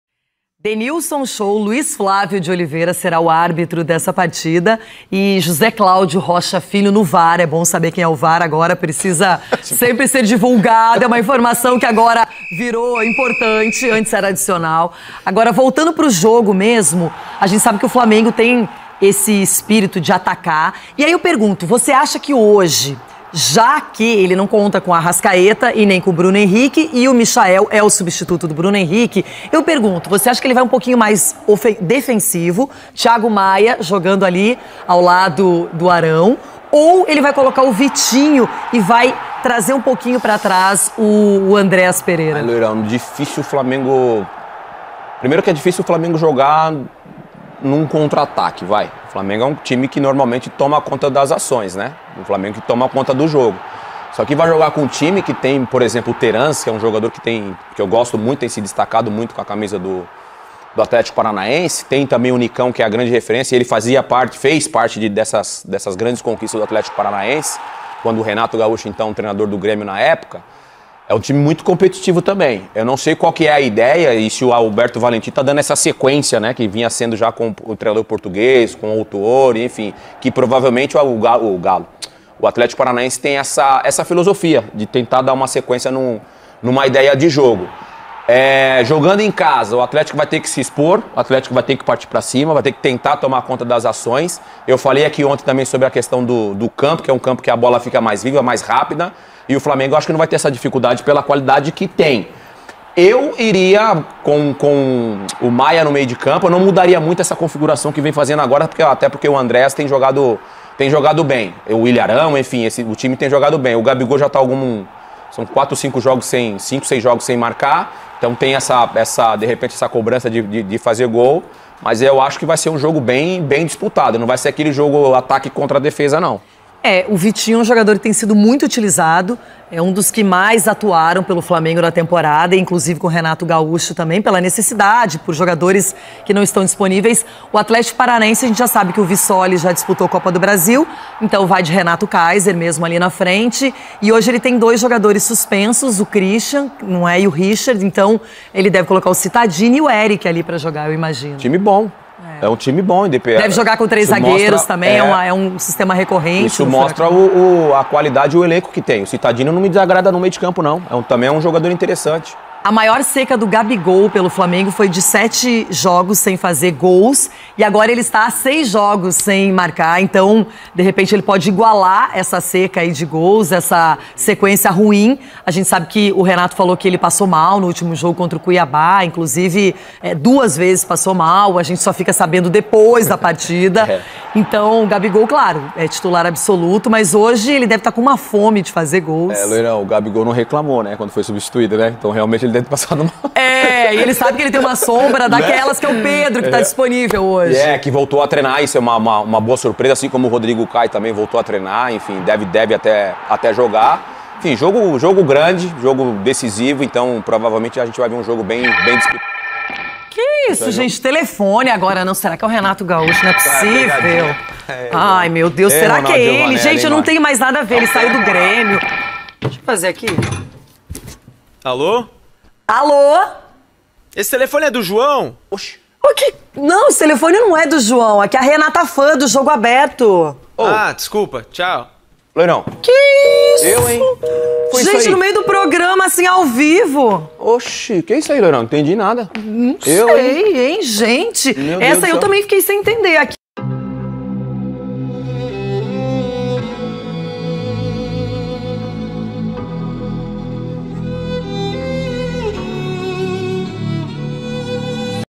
VAI SER MALVADÃO? FLAMENGO ENFRENTA O ATHLÉTICO-PR EM BUSCA DA FINAL
VAI SER MALVADÃO? Flamengo enfrenta o Athlético-PR em busca de mais uma final da Copa do Brasil. Renato Gaúcho terá desfalques. Confira!